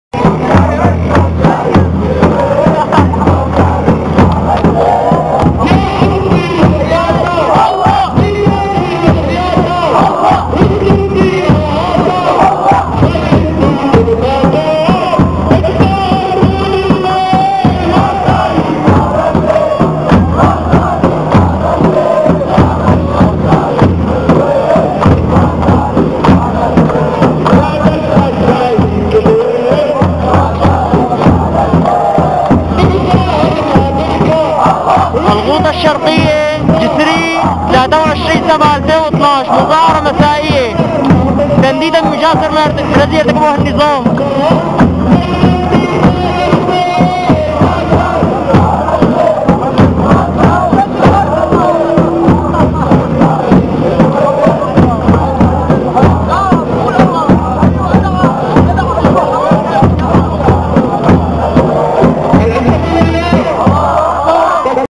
شام ريف دمشق جسرين مسائيات الأحرار نصرةً ...
شام ريف دمشق جسرين مسائيات الأحرار نصرةً للجيش الحر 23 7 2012